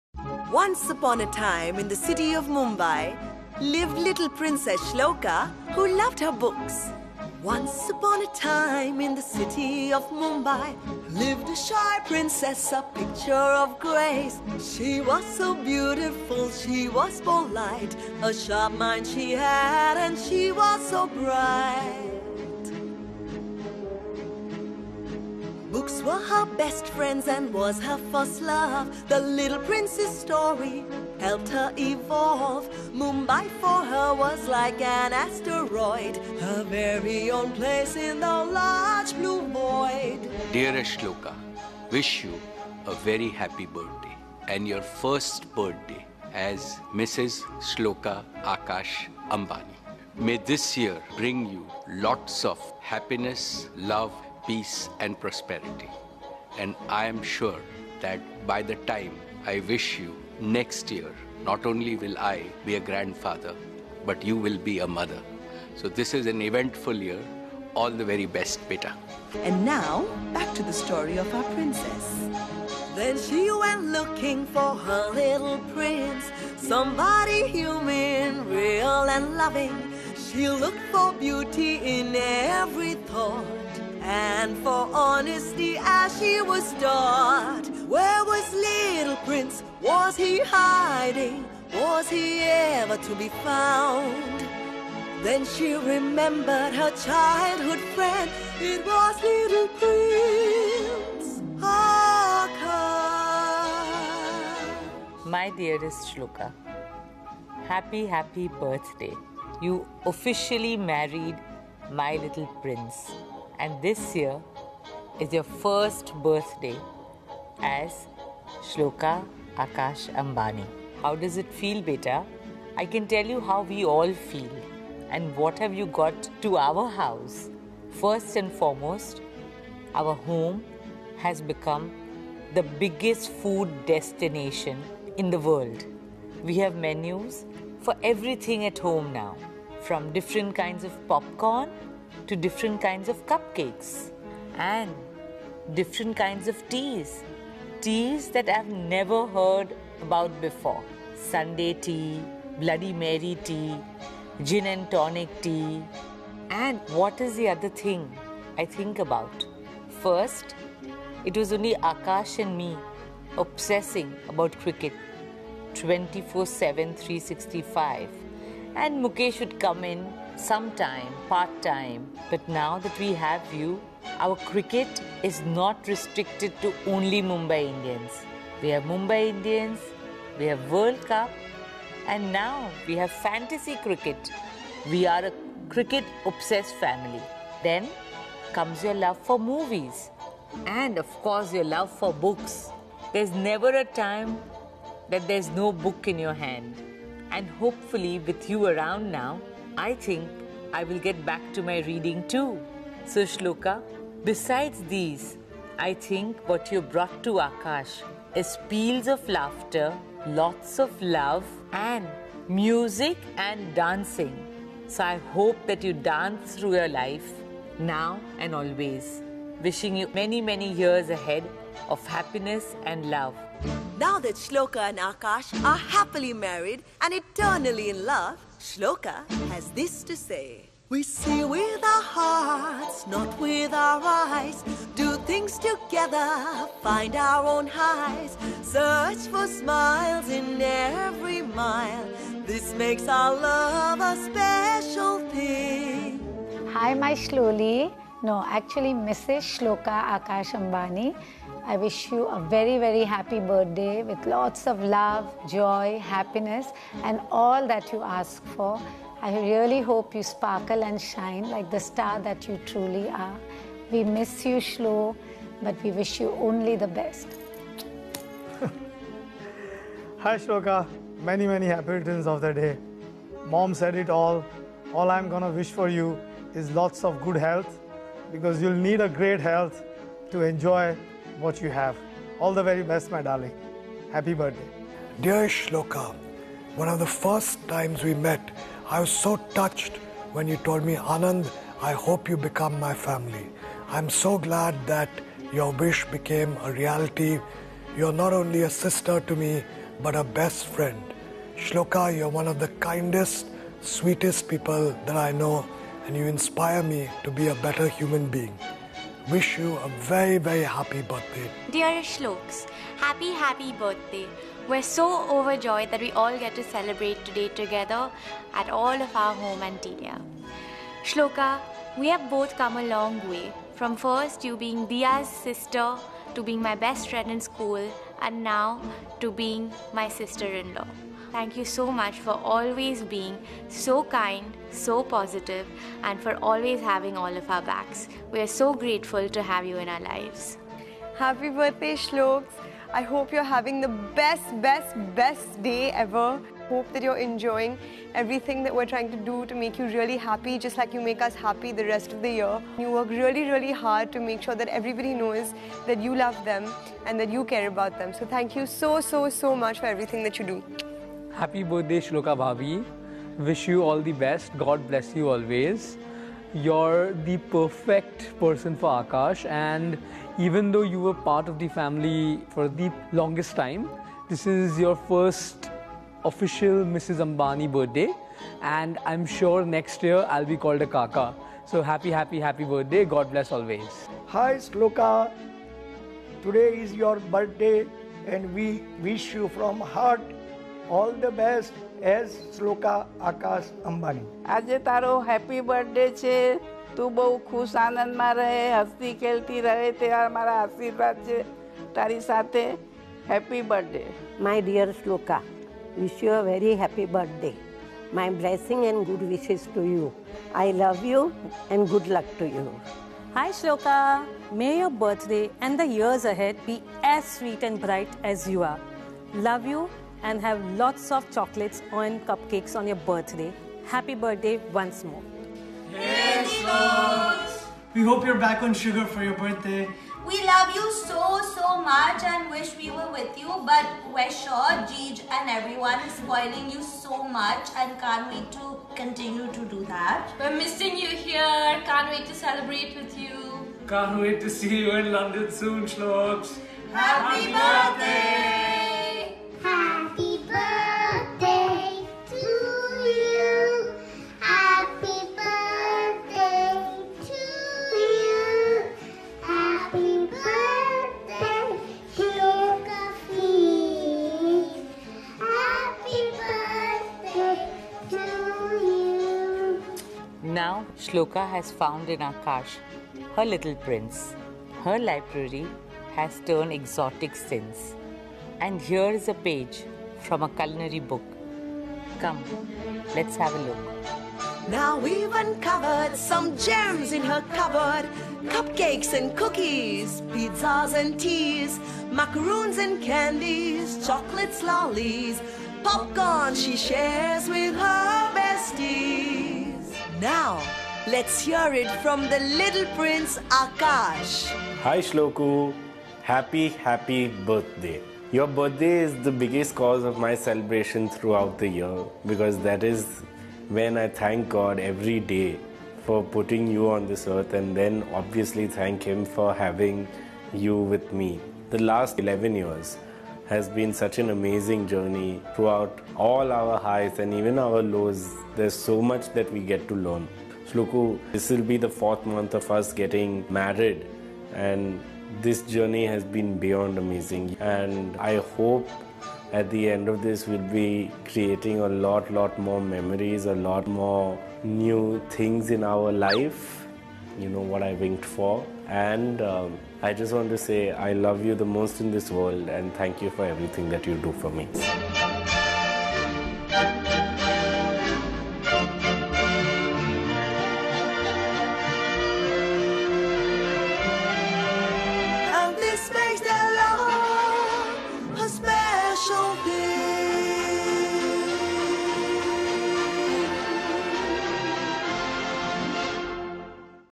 Shloka Ambani gets adorable birthday gift from family
Reliance Chairman Mukesh Ambani and Nita Ambani's son Akash Ambani tied the knot with diamantaire Russel Mehta and Mona Mehta's daughter, Shloka Mehta five months ago in Mumbai. The family is all set to celebrate the birthday of the newest member of their family Shloka Akash Ambani. And to celebrate her first birthday as an Ambani bahu, the family got together to wish her in an adorable video.